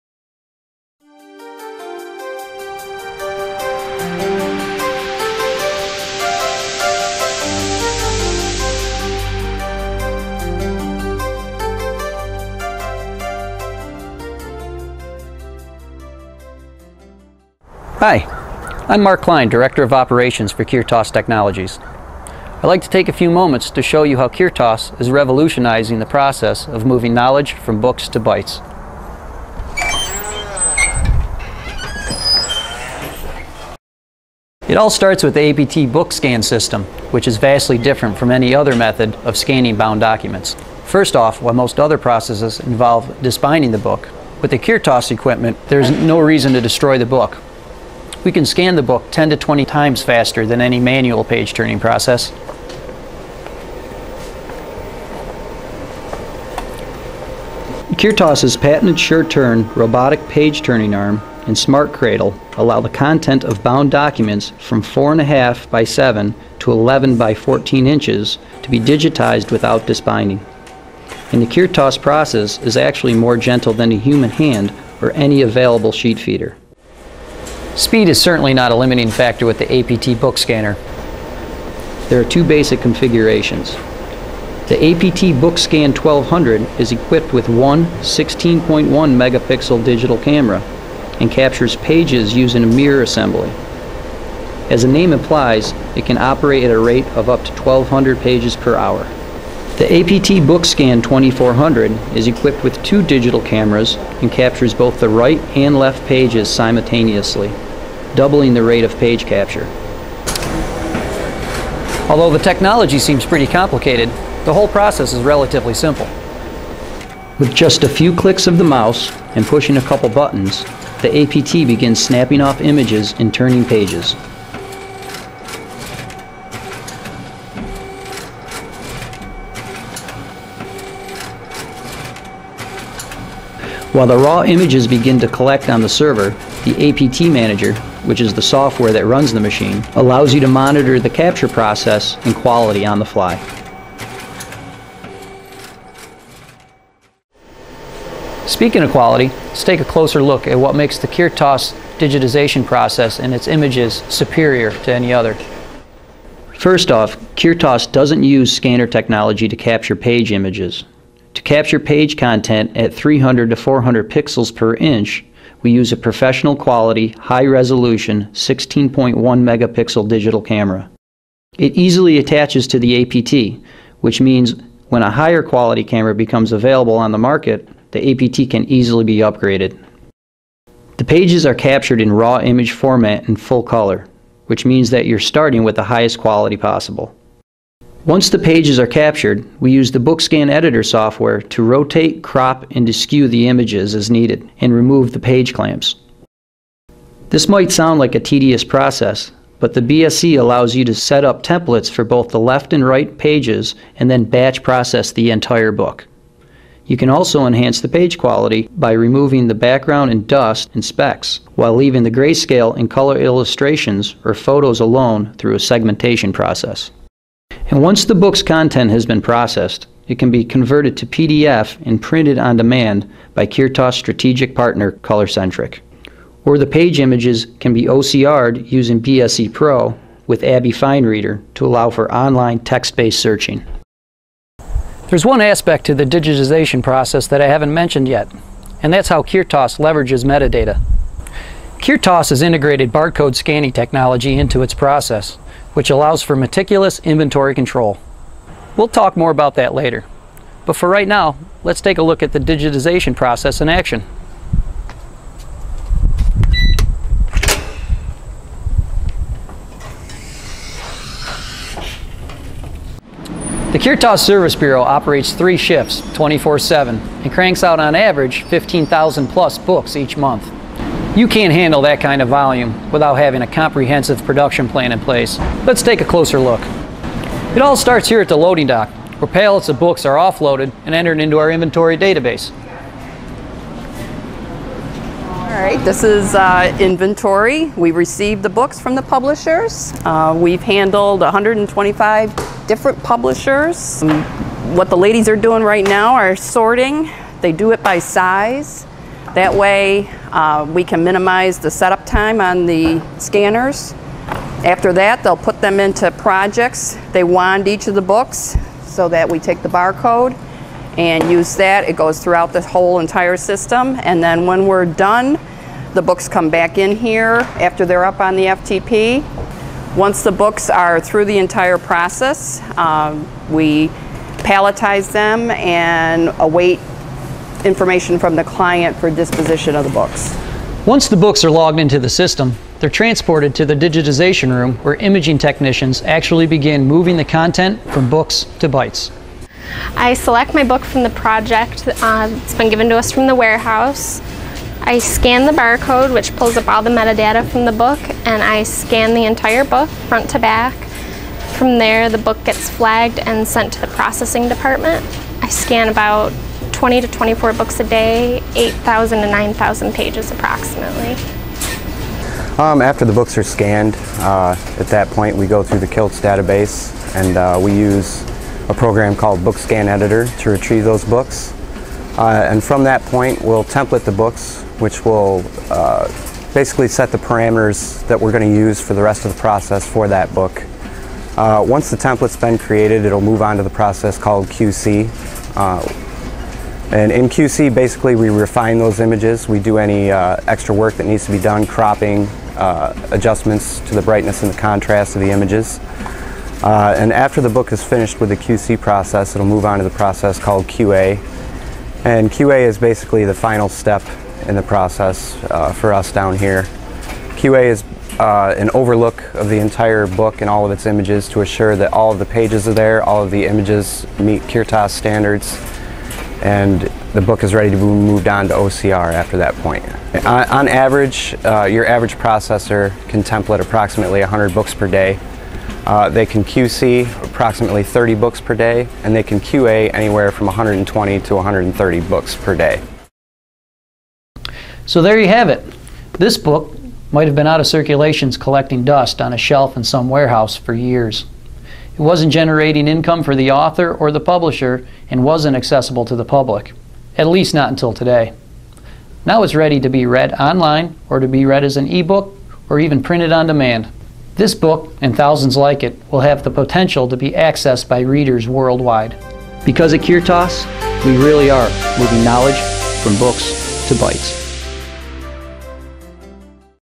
Book Digitization: A Kirtas Visual Tour
A tour of the book digitization firm Kirtas Technologies. The film gives an overview of the process of digitizing books for libraries and other institutions.